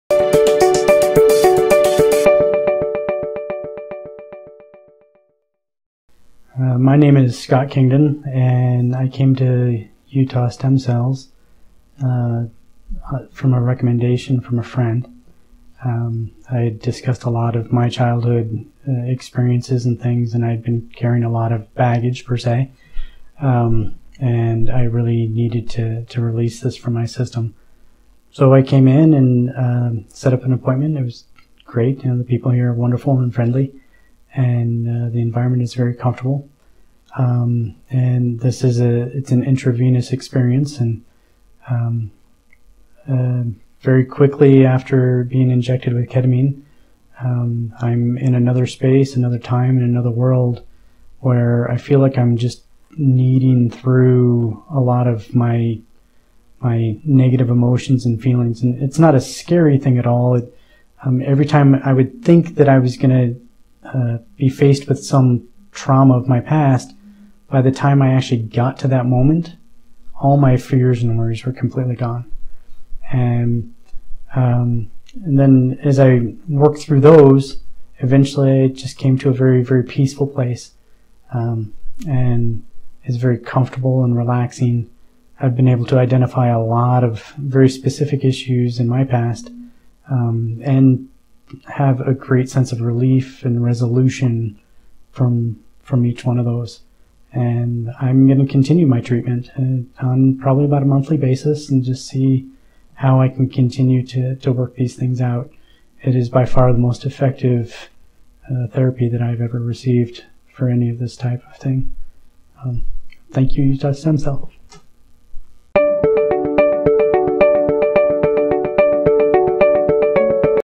Depression / Anxiety / PTSD - Ketamine Treatment - Utah Stem Cells
Scott Kingdon, a 42-year-old male
Depression / Anxiety / PTSD
Ketamine Treatment

by Dr. Bill Cimikoski